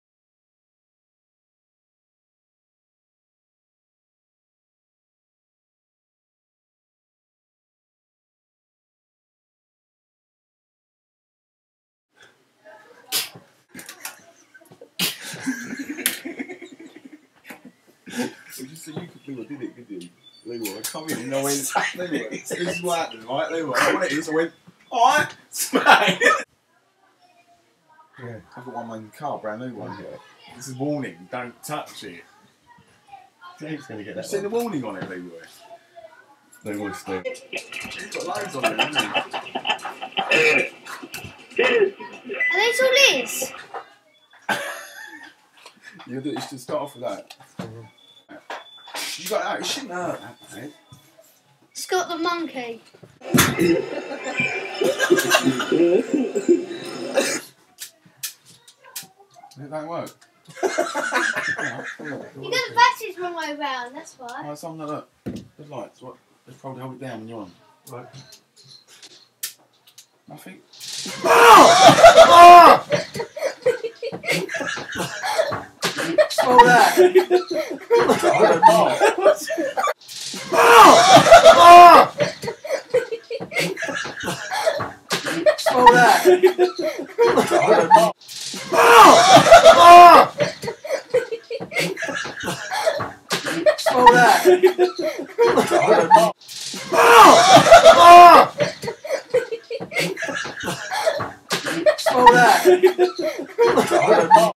Leroy's Shocking Shock
This Clip will make you wet yourself.

How not to use a electric fly zapper.

Please leave a rating and i you like send it to your friends